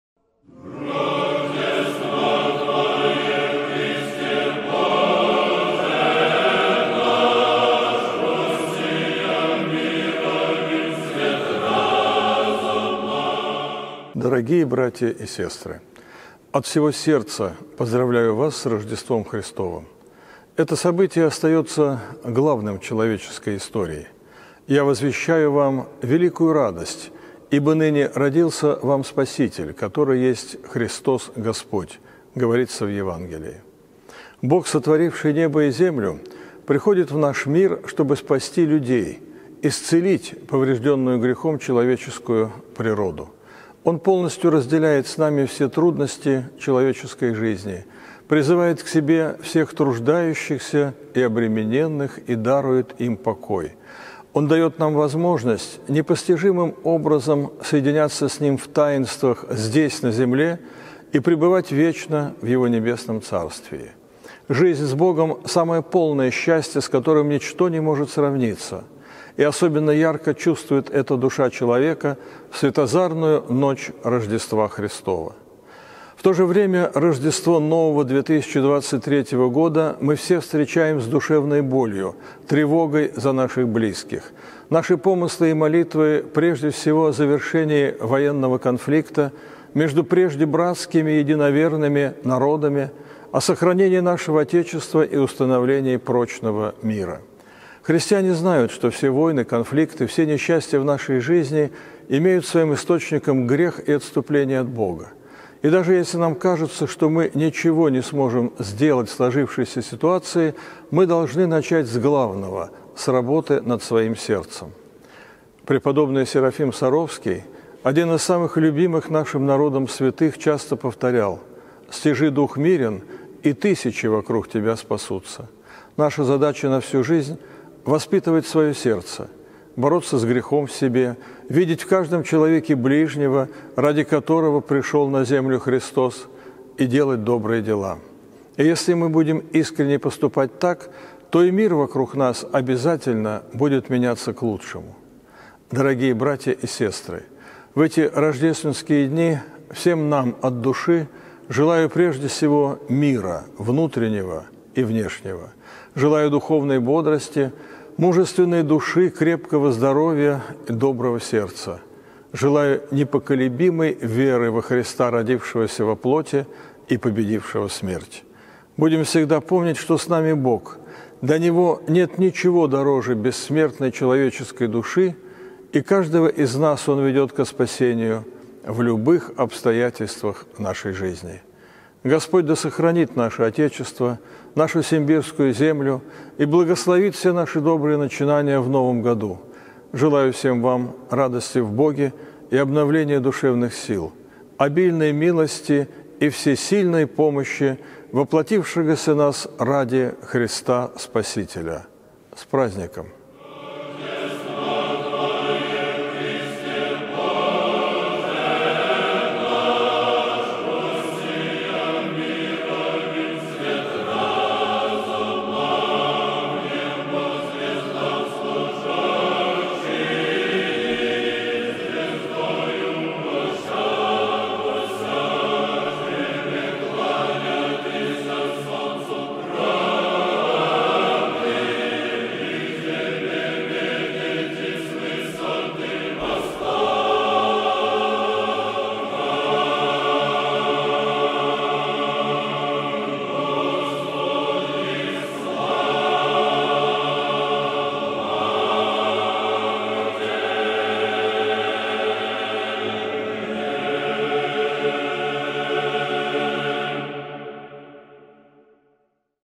Владыка Лонгин: «С Рождеством, дорогие братья и сестры!»
Митрополит Симбирский и Новоспасский Лонгин поздравляет православных ульяновцев с Рождеством Христовым.